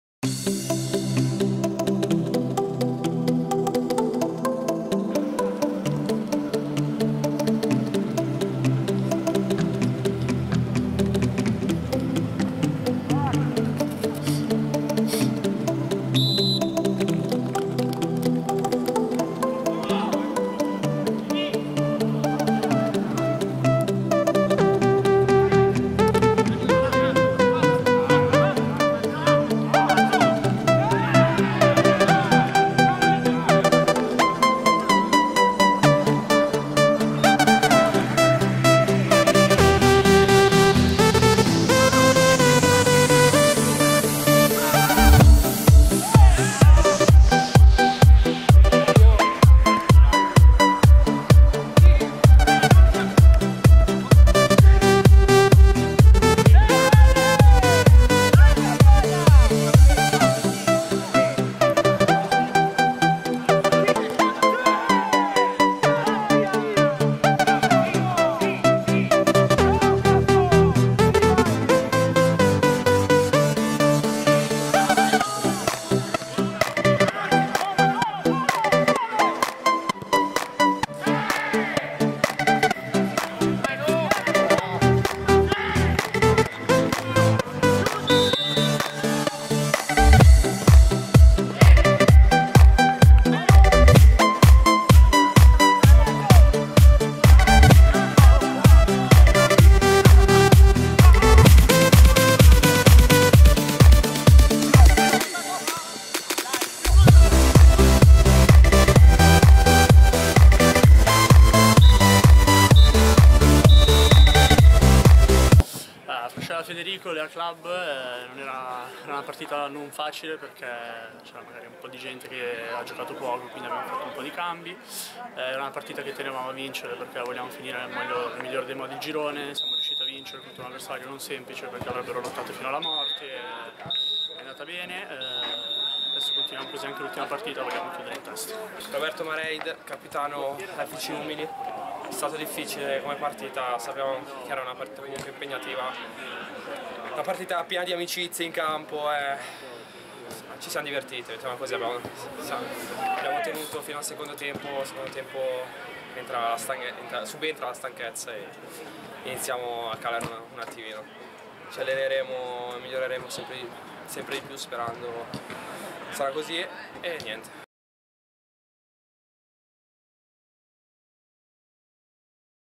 Fascia A - Girone B: LEA Club - FC Umili
Prima Fase - Girone A - 6a giornata

Masseroni Marchese 1 -15/01/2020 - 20:00

 LEA Club - FC Umili 7-3

LEA Club: Lella (GK), Rebuscini, Venanzetti, Accetta, Semenzato M., Lettieri, Blandini, De Chirico, Raganato, Frascella, Vaini, Fautario.

FC Umili: Carlin (GK), Tito, Cicogna, Stanzione, Capobianco, Mareid, Carbone, Semenzato O., Vallesi, Pascon, Galletta.

Reti: st. 6' e 11' Vallesi (UM); 8' Frascella (LEA); 10' Accetta (LEA); 14',15', 17' e 21' Lettieri (LEA); 20' Galletta;  21' Vaini (LEA)